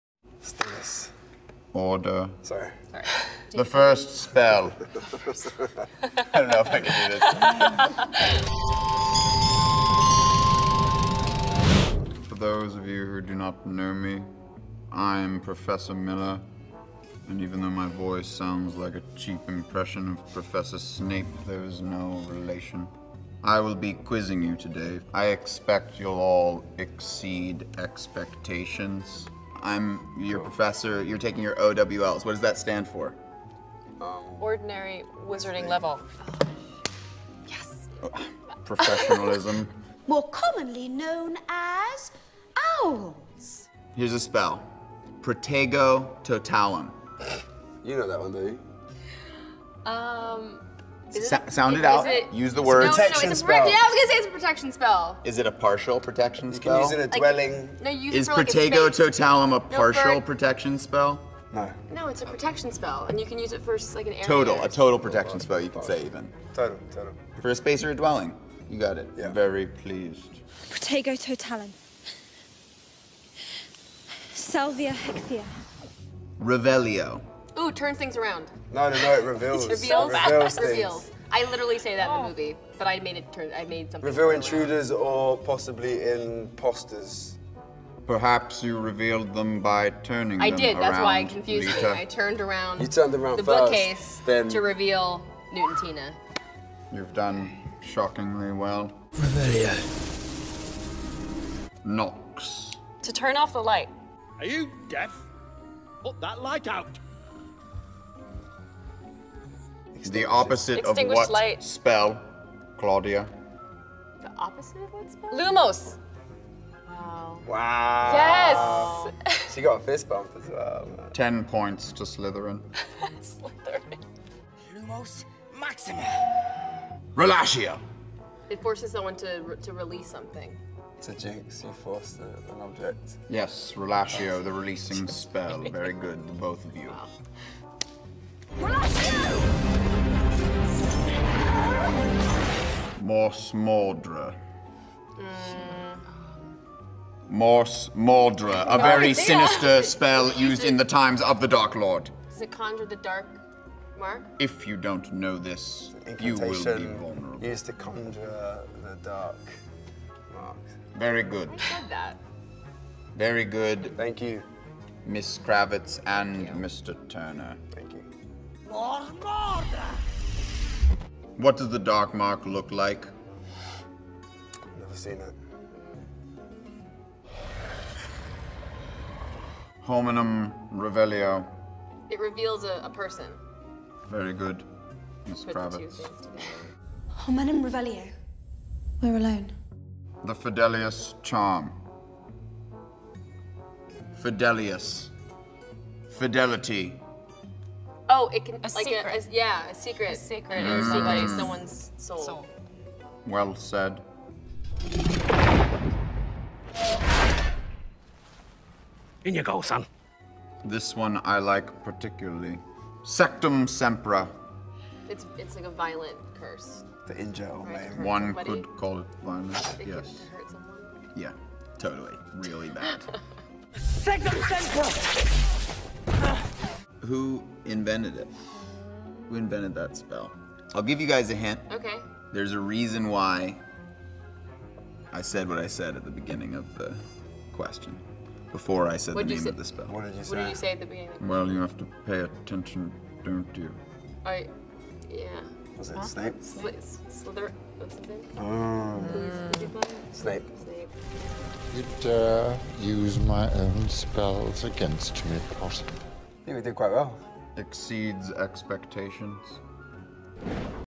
[자막] 신동범 출연진들은 해리포터 주문을 얼마나 잘 알고 있을까?(에즈라 밀러, 클라우디아 킴, 칼럼 터너, 조이 크라비츠)
-자막 설정 켜고 보세여
-자막 오역의역 엄청 많을 것입니다
-저는 영어 못하는데 목마른 사람이 우물 파는 마음으로 만든 것이에요. 그러니 여러분들의 많은 지적과 도움이 필요합니다. 집단 지성의 힘 믿습니다. 에즈라 밀러 덕질. 혼자... 어렵다..▶▷이제는 혼자가 아닌 것 같아요♬